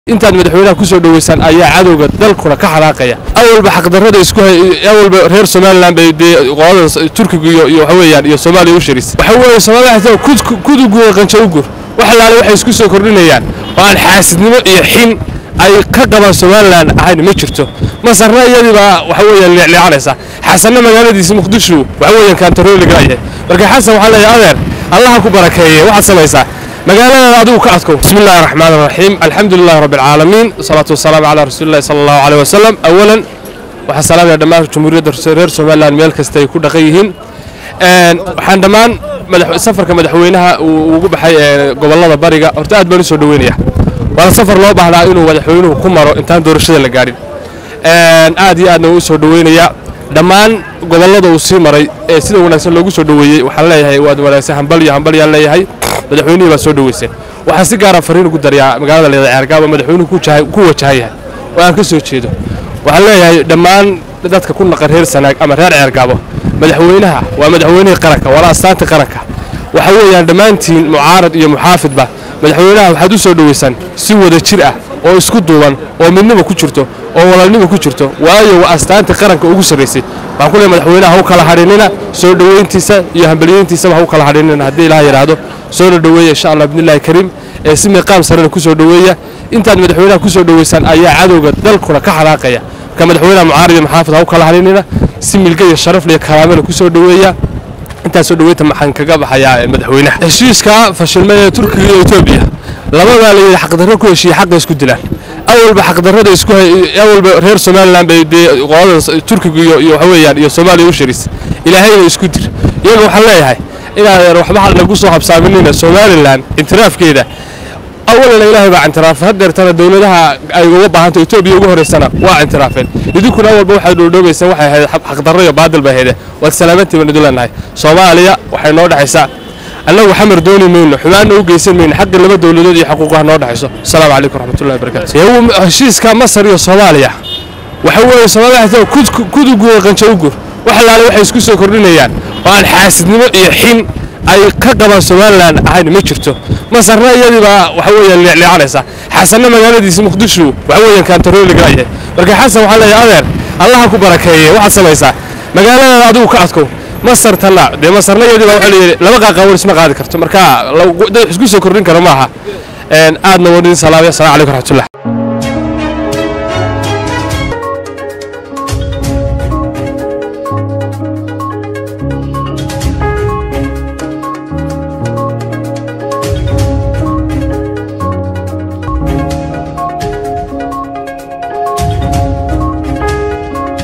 Cadowga Somaliland Wuxuu Dusha Ka Eegayaa sida aad Madaxwaynaha U soo Dhawaysaan
Raandhiis Media Kala Soco Wararkii u danbeeyey ee Soomaalida